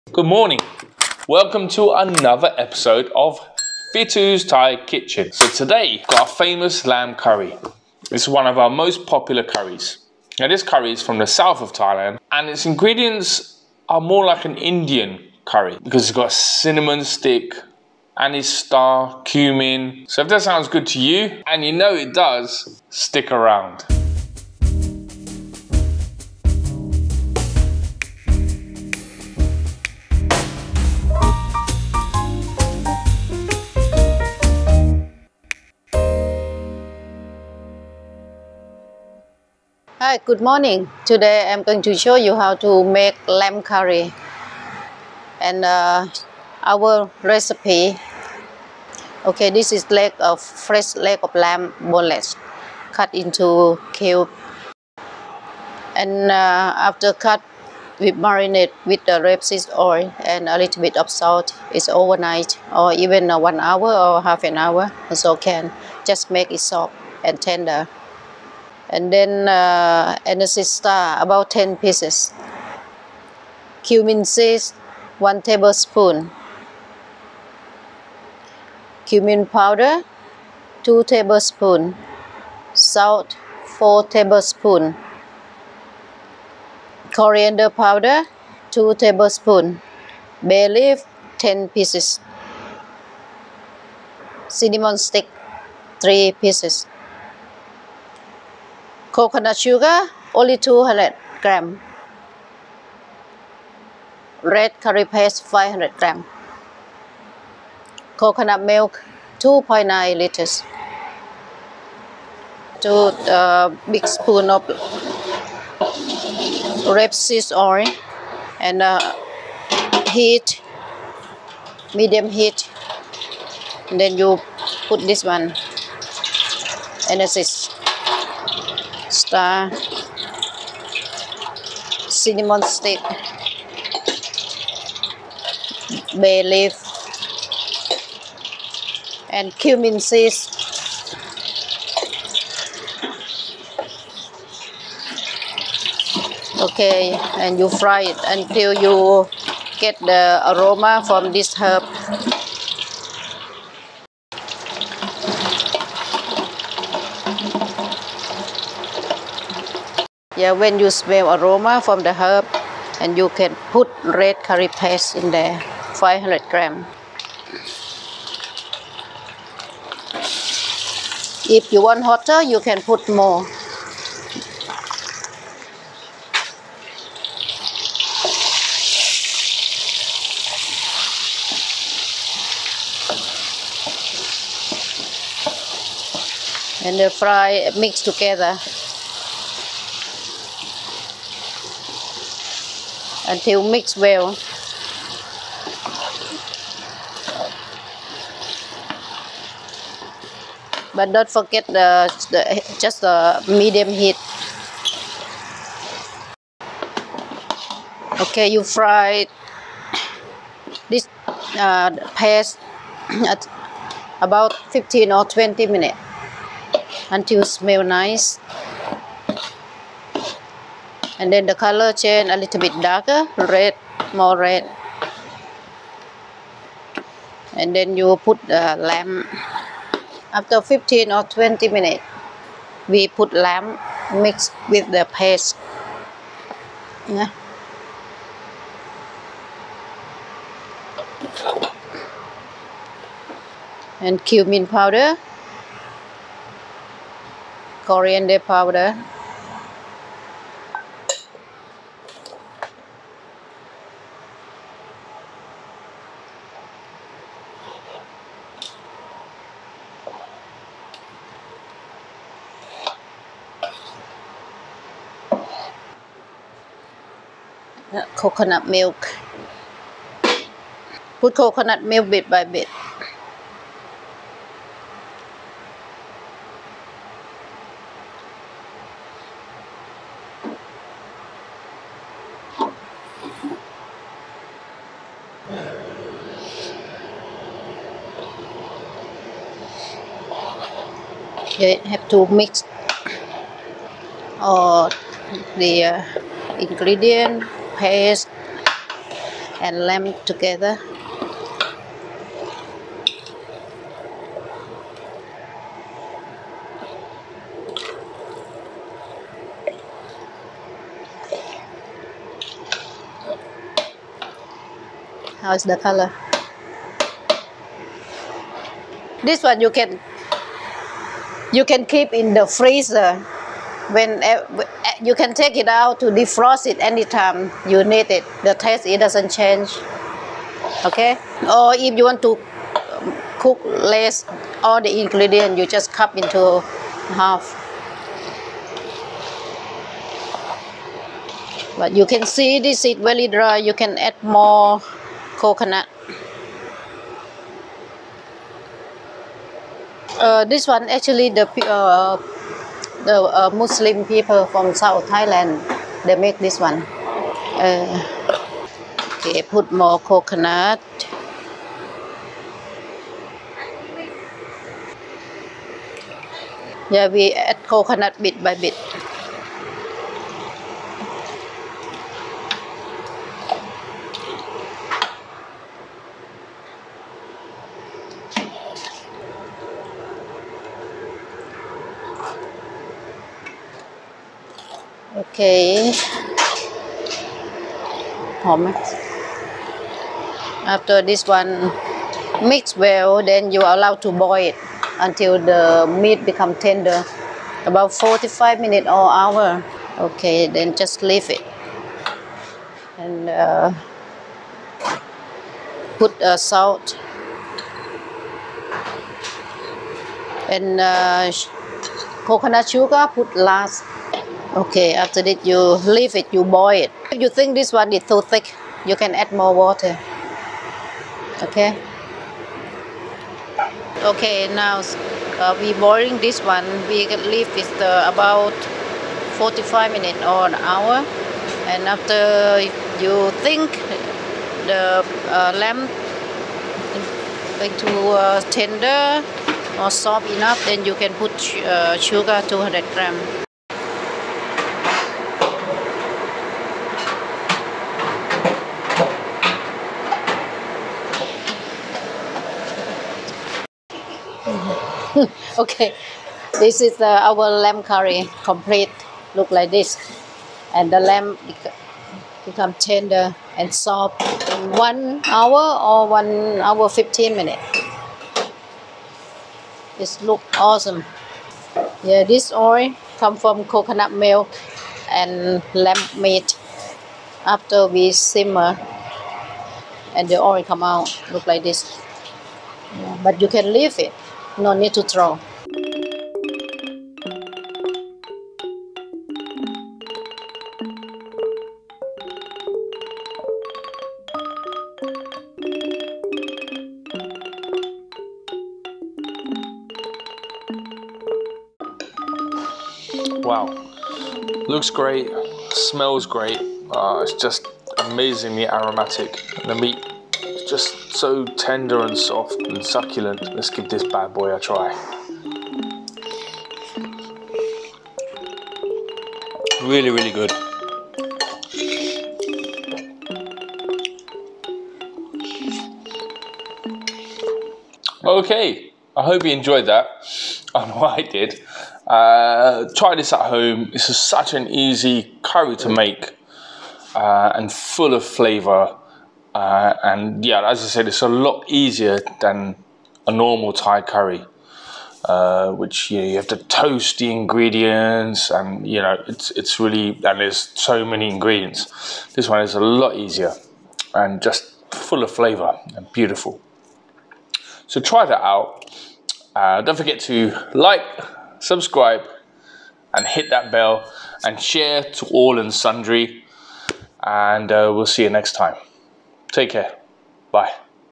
Thai lamb curry recipe แกงเนื้อแกะ Southern Thailand Lamb Curry
Thai lamb curry recipe
แกงเนื้อแกะ Southern Thailand Lamb Curry

This is the curry that keeps you coming back!
Simple, fresh, and packed full of flavour. A favourite in our restaurant for many years.
Simple tutorial with Chef Pi Tou showing us how to prepare the various ingredients for Thai Lamb Curry.
This curry is from the south of Thailand where there are a lot of Muslim communities that have ancestry from many different countries. Generally Lamb is not a popular meat in Thailand, they don't like the smell or the flavour, but in the Muslim communities they love it, their curries tend to have more Indian ingredients plus Thai ingredients making them really quite special. This curry looks like massaman curry but isn't massaman, this curry uses red curry paste.

Here's the recipe:
4kg Leg of lamb cubed and marinated in rapeseed oil and salt
10x Anis stars
1 tbsp Cumin seeds
2 tbsp Cumin powder
2 tbsp coriander powder
10x bay leaves
3x cinnamon sticks
200g coconut sugar
3 large tablespoons table salt
500g red curry paste (Nittaya red curry paste is good)
2.9 litre Aroy-D coconut milk

Chapters
00:00 Killan tells us about the video
00:24 intro
00:39 ingredients
01:52 in the pan
02:47 add the curry paste
04:00 adding the lamb, cumin and coriander powder
04:30 add coconut milk
06:40 add salt
07:00 boil the curry sauce
07:37 add coconut sugar
08:22 taste test
08:47 outro

As with all Asian cooking this is a rough guide with regards to amounts, adjust for your own taste and spice tolerance.
If you want to make it spicier then toast birds eye chillies instead of large sweet chilli.
Good luck and let us know how you get on.